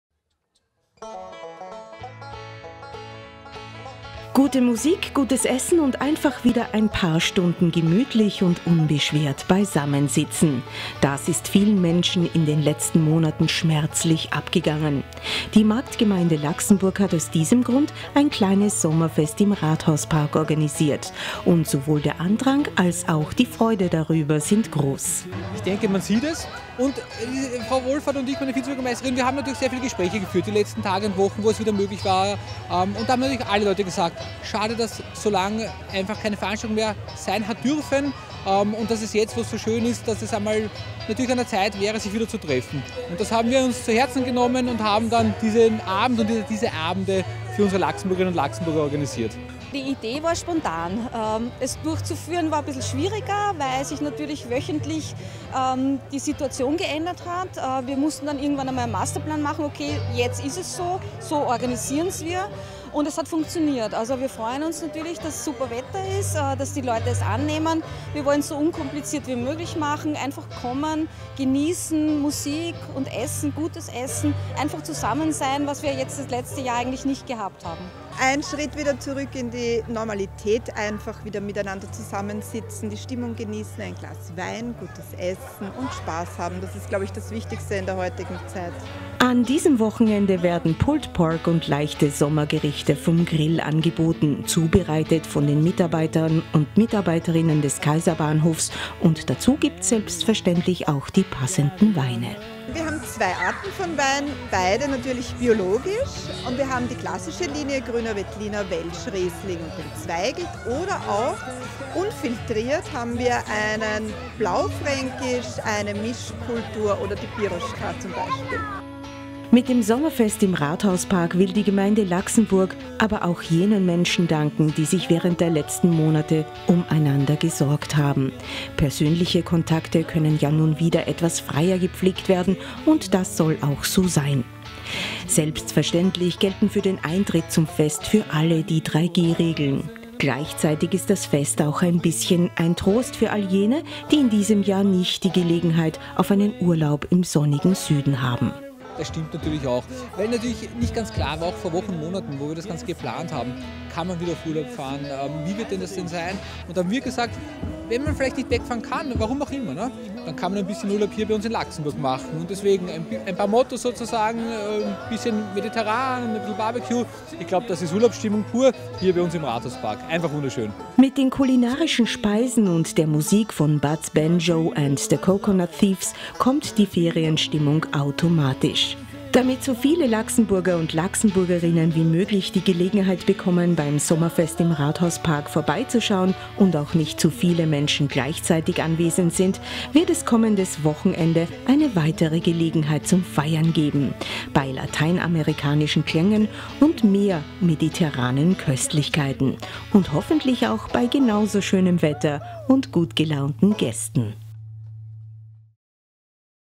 Laxenburg . Sommerfest im Rathauspark
Gute Musik, gutes Essen und einfach wieder ein paar Stunden gemütlich und unbeschwert beisammen sitzen. Das ist vielen Menschen in den letzten Monaten schmerzlich abgegangen. Die Marktgemeinde Laxenburg hat aus diesem Grund ein kleines Sommerfest im Rathauspark organisiert – und sowohl der Andrang, als auch die Freude darüber sind groß.